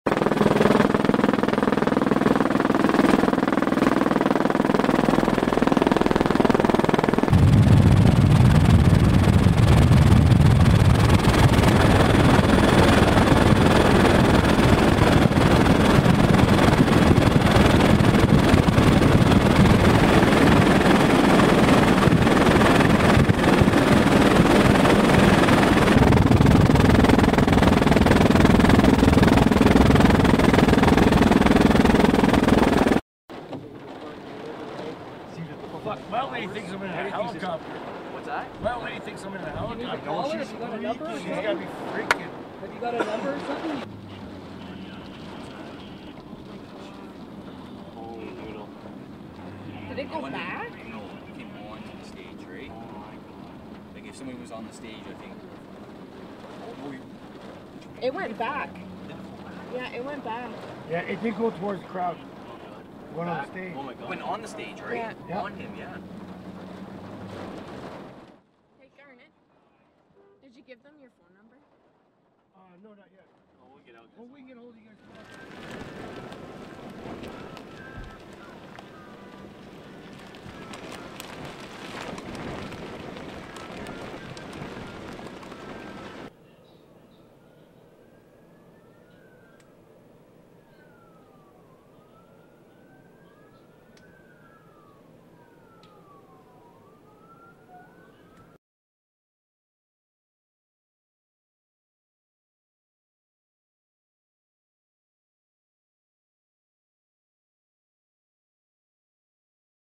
Big Valley Storm - Pre and Post
iMediatv.ca was shooting stuff for KGCountryTV.com at Big Valley Jamboree. We were up in the sky with Gemini Helicopters moments before the fast moving storm ripped through the area.  Luckily due to the fast thinking Pilot we landed safely and returned to the site to find a tragic scene.  I am posting this to show the magnitude of the storm not to dwell on this tragedy.  Our prayers are with those who were hurt and killed in the storm.

Will be reloading a better quality video shortly...of the footage...sorry for the grainy images.